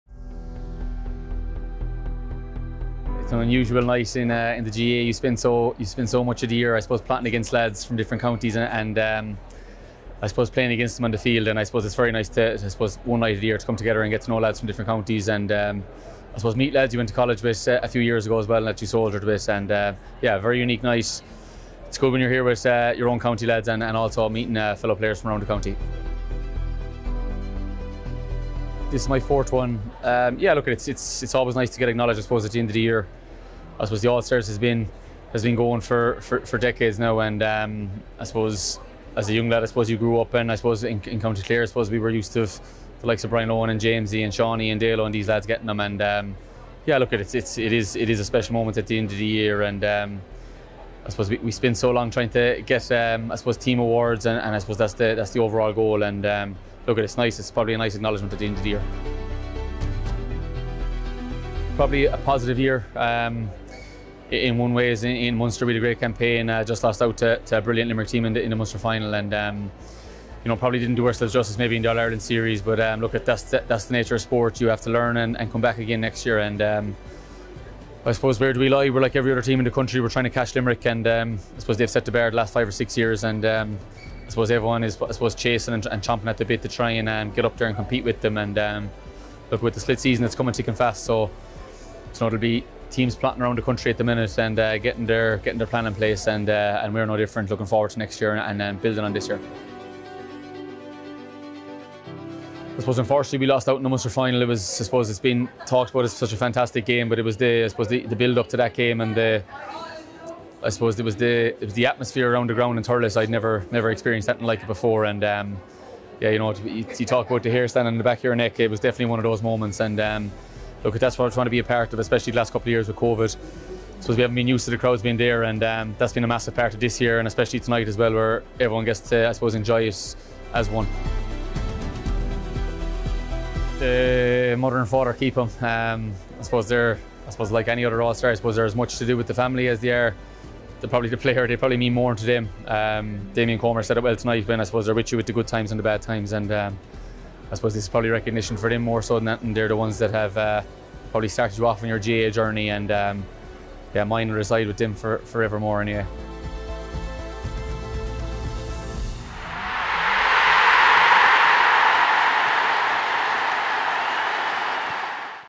2022 PwC All-Stars Awards: Clare's Tony Kelly
'It is a special moment at the end of the year...'- GAANOW spoke to Clare and
Ballyea's Tony Kelly as he received his fourth PwC All-Star Award in Hurling at the 2022 PwCAllStars Awards!